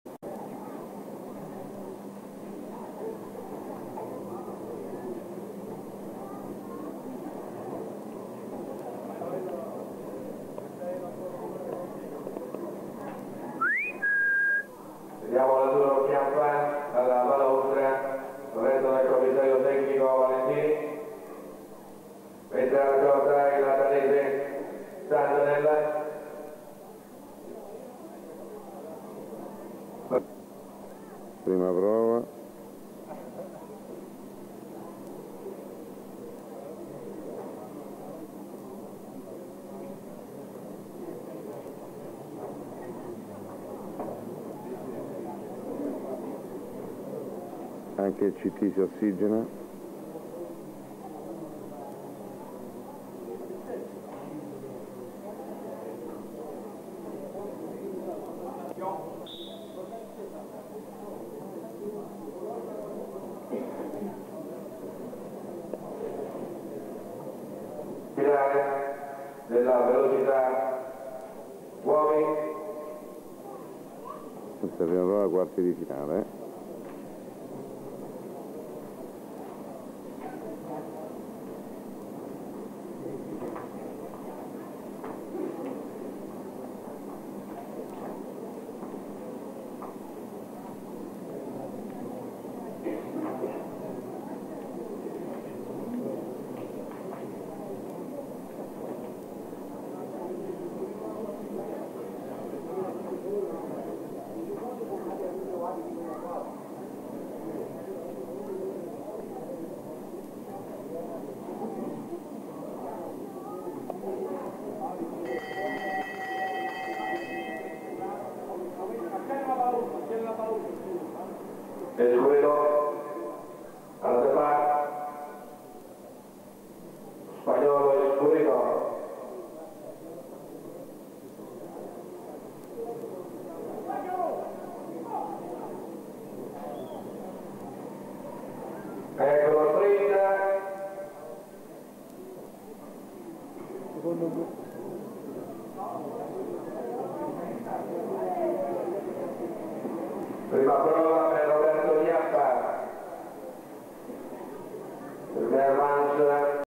sprint world cup 1994 in Italy, Chiappa vs Dandanell
Roberto Chiappa vs Brian Dandanell ,Bassano del Grappa 1994